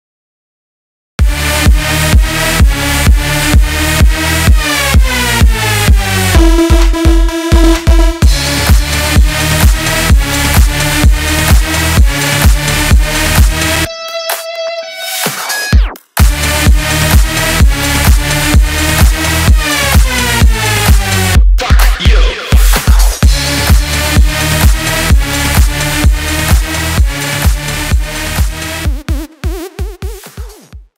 PREVIEW - BTS: KRYOMAN vs STELLAR - World Tour 2013 | Four One Ent
Preview of an upcoming video going Behind the Scenes of Kryoman vs Stellar - Vegas MDW 2013.